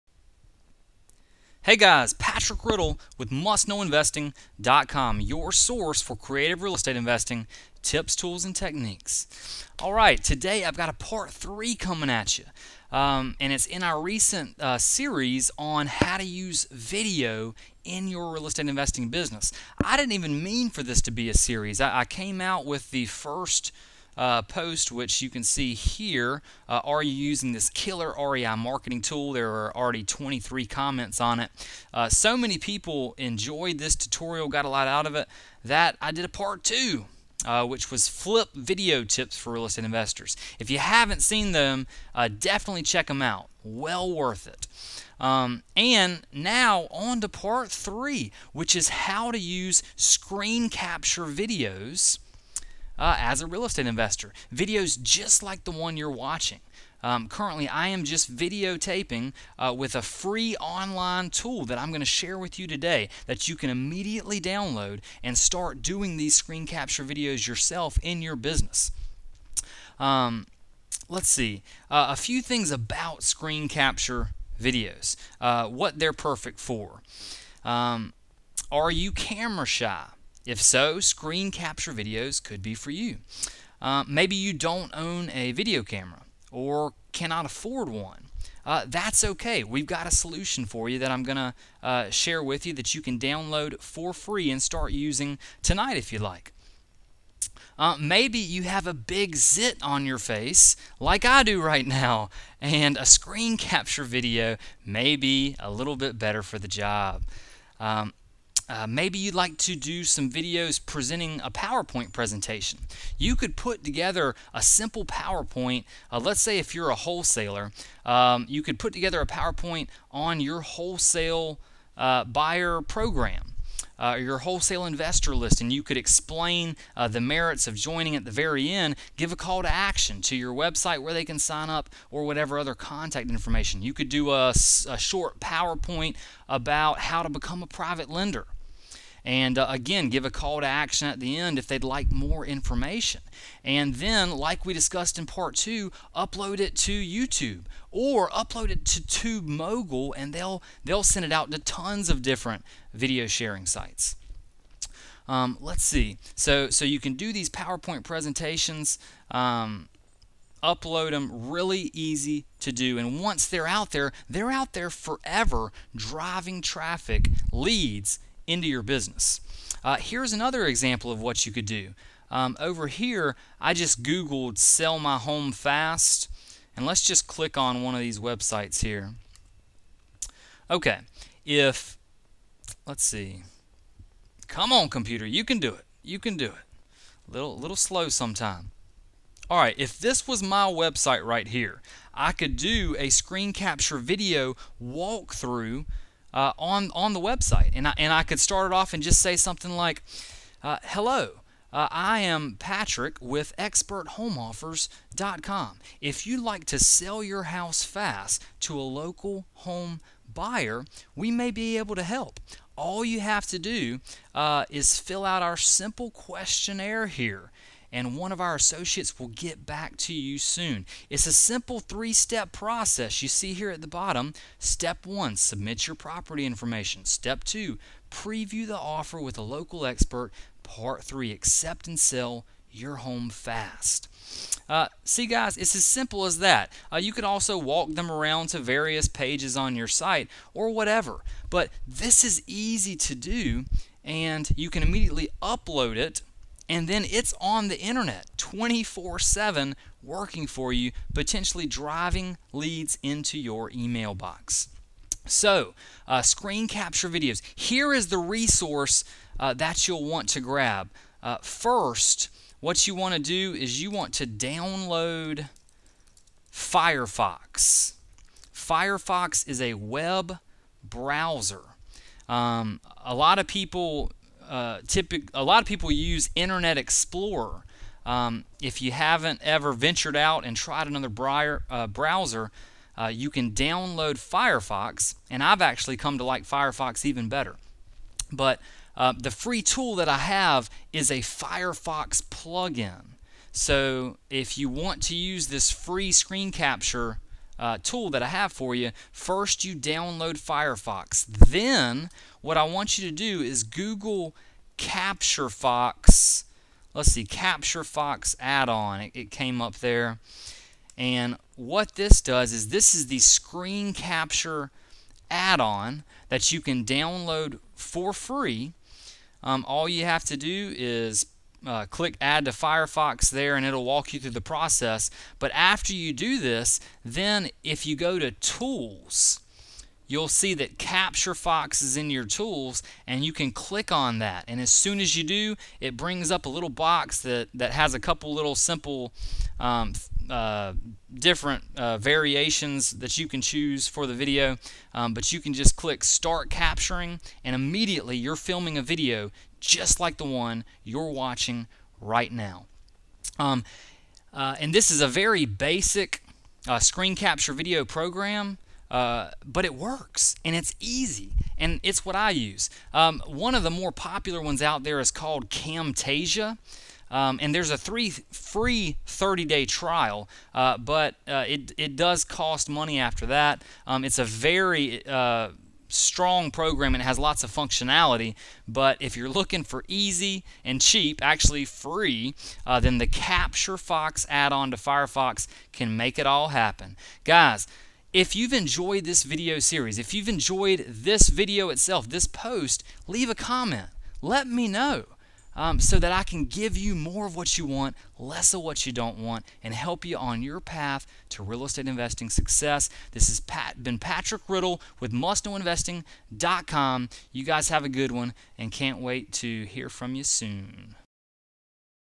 How to Create (FREE) Screen Capture Videos for Your Real Estate Investing Biz (Part 3)
As a real estate investor, marketing will make or break you. And video marketing is absolutely HUGE right now.

In this video (which is Part 3 of a series on video marketing in real estate investing), you'll learn how to use screen capture videos to get buyer, seller, tenant, heck ... even private money leads flowing into your REI biz.

Plus, I share a killer tool that you can download for FREE to do your screen capture videos.

~ Patrick
... aka P-Rid

P.S. - Leave a comment before you go. Have you enjoyed this series? Tell me how I can help ...